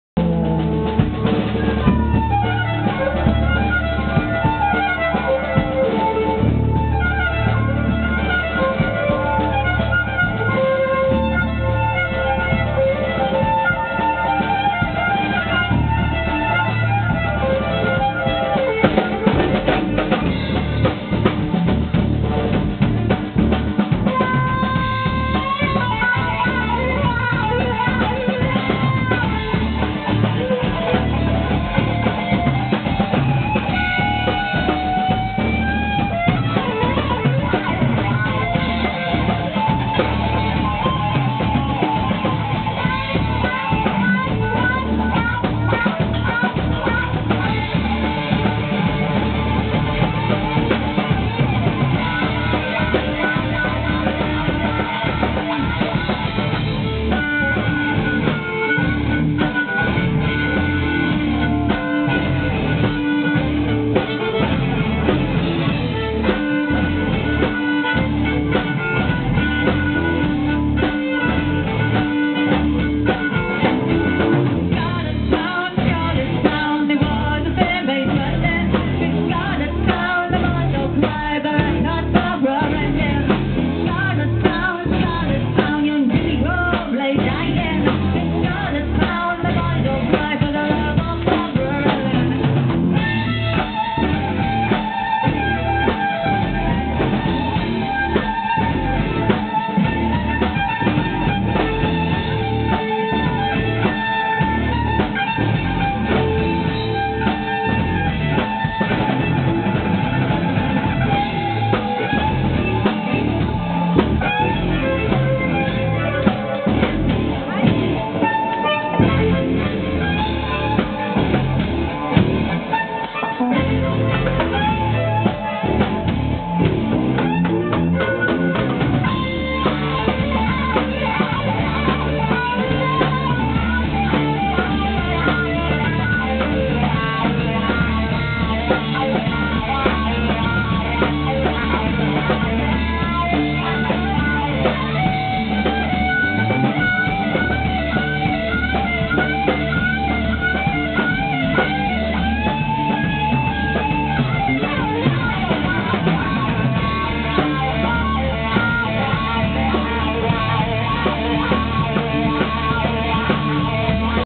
Bluehorses Folk on the Water 2004
Fantastic Bluehorses Band from Wales, having a rest from touring this year ( shame ) so uploaded this for all you BH fans.  Enjoy.  Cant imagine why they have never played at Cropredy FConvention R.
Toad